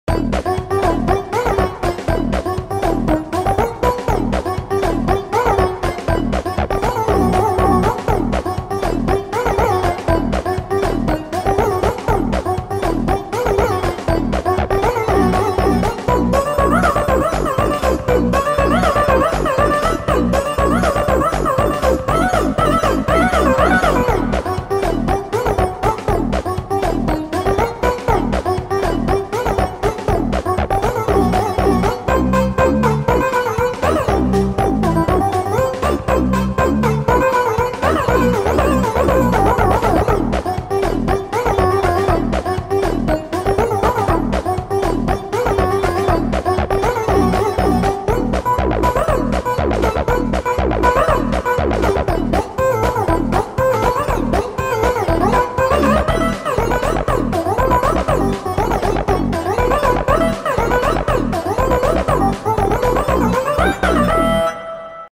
I made Music that Sounds like an Angry Dog
Check out this fun, upbeat synth music inspired by Keri Koyuki from the anime: Boku no Tonari ni Ankoku Hakaishin ga Imasu! I made music that sounds like an angry dog - a unique and quirky tune to add to your playlist.

Other Tools:
Wondershare Filmora
Avee Player
Canva
Sora AI Image Generator
Fl Studio